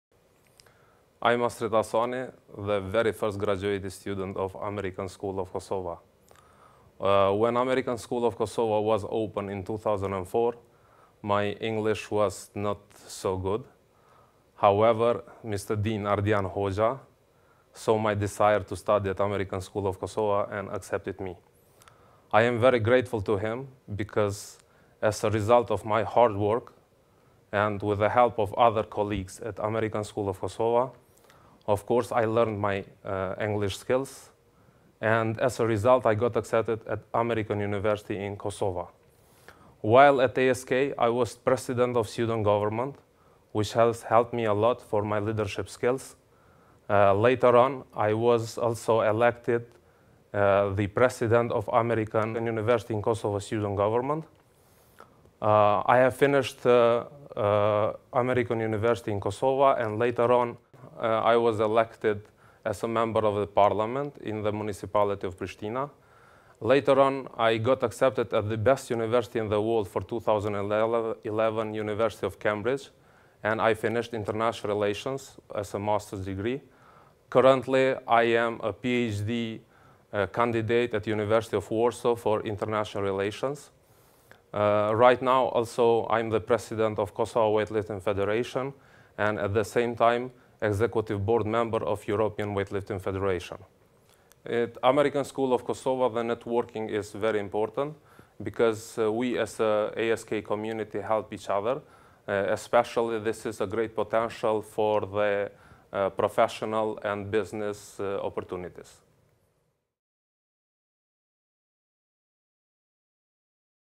Proud ASK Alumnus | Astrit Hasani
Astrit Hasani, currently Chairman of the Kosovo Weightlifting Federation, shares his experience as a proud ASK Alumnus